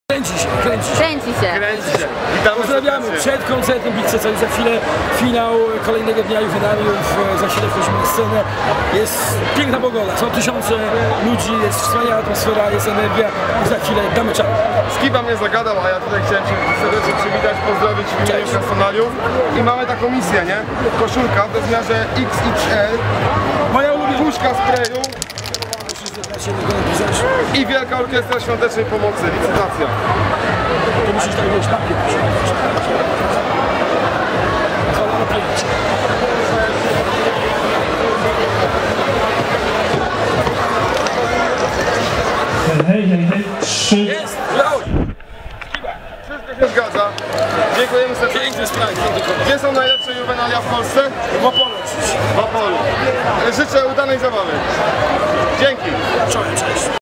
Banan na Piastach - Krzysztof Skiba (Big Cyc)
Banan spotkał się na backstage z Krzysztofem Skibą z zespołu Big Cyc! Zobaczcie co ma Wam do powiedzenia!!!